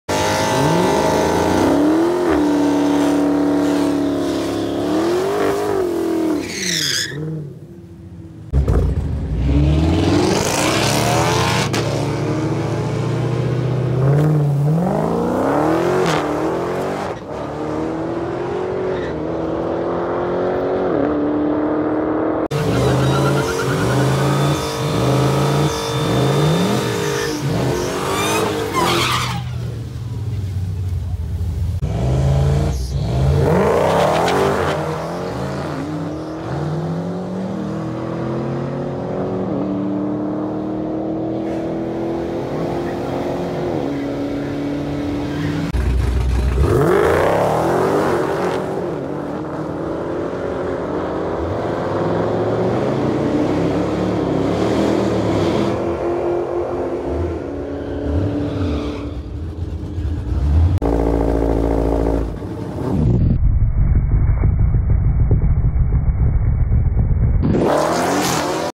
Dodge Demon killing it at the track! Grandpa owned Dodge Demon!!!
Heres a few clips on the new Dodge Demon!

Also follow @the_red_list_group for future event!